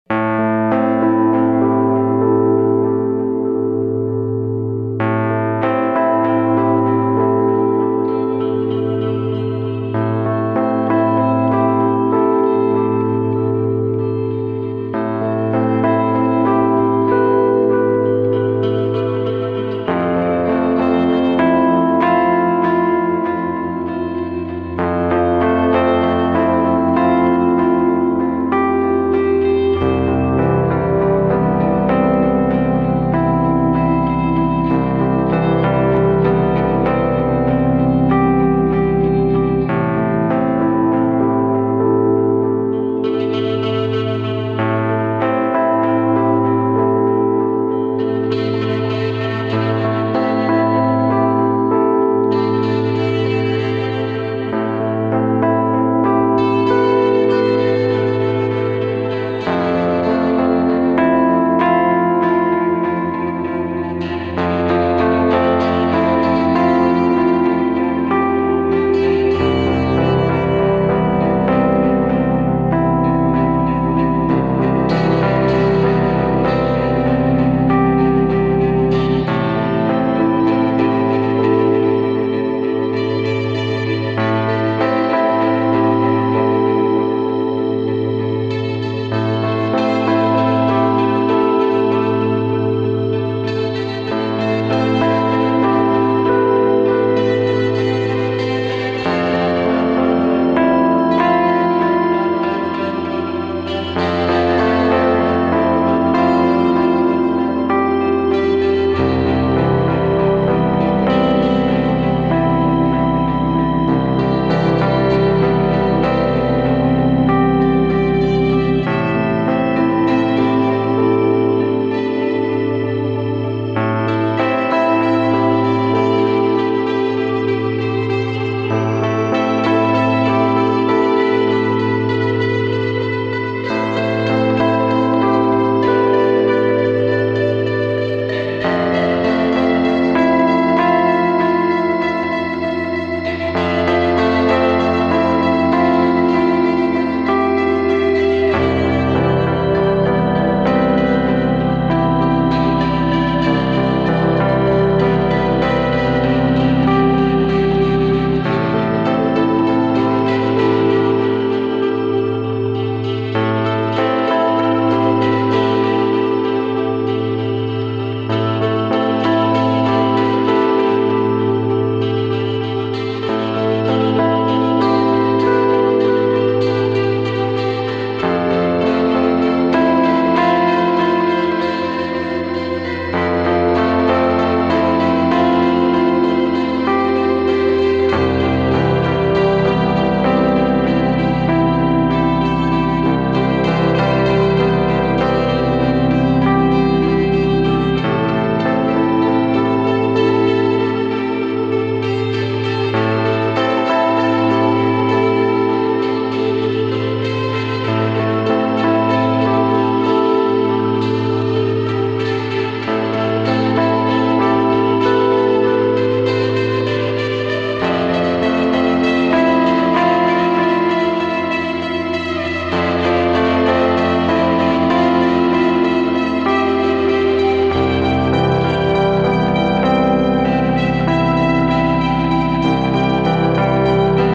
Gengar time lapse
Here’s a time lapse of my digital painting for one of my favourite Pokemon, the master of shadows and terrifying smiles.